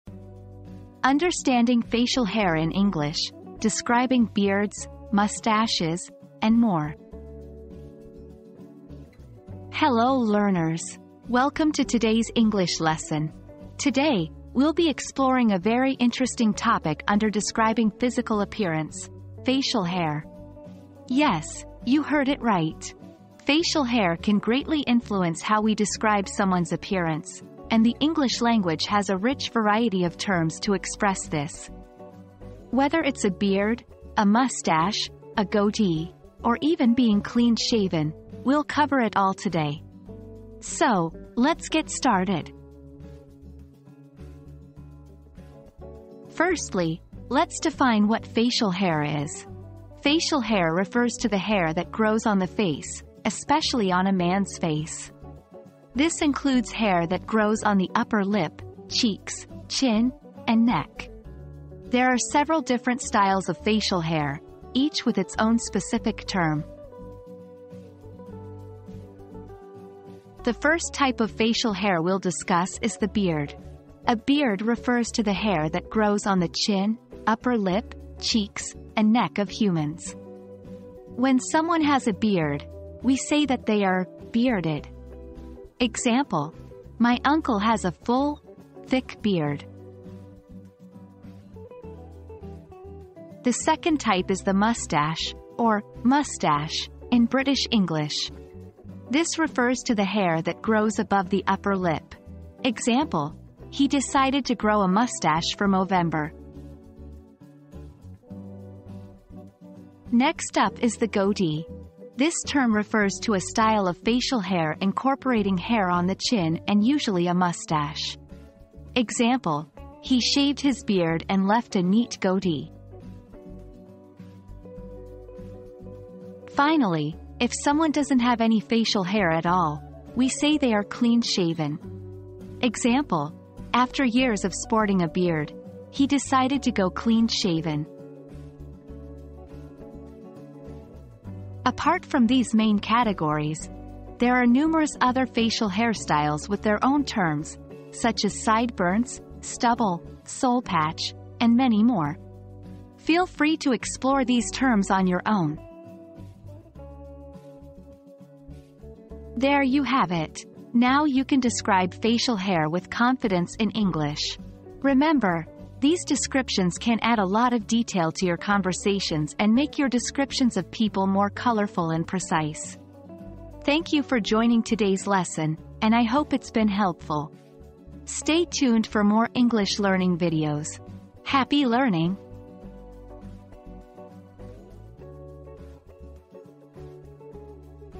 Understanding Facial Hair in English: Describing Beards, Mustaches, and More
Facial Hair 101: Describing Beards, Mustaches, and More • Discover the fascinating world of facial hair and learn how to describe different styles like beards, mustaches, and goatees. From the basics of facial hair to the concept of being clean-shaven, this video covers it all. Join us for an entertaining and informative lesson on understanding facial hair in English!

00:00 • Introduction - Understanding Facial Hair in English: Describing Beards, Mustaches, and More
00:45 • Understanding Facial Hair
01:09 • The Beard
01:32 • The Mustache
01:49 • The Goatee
02:05 • Clean-Shaven
02:21 • Other Facial Hair Styles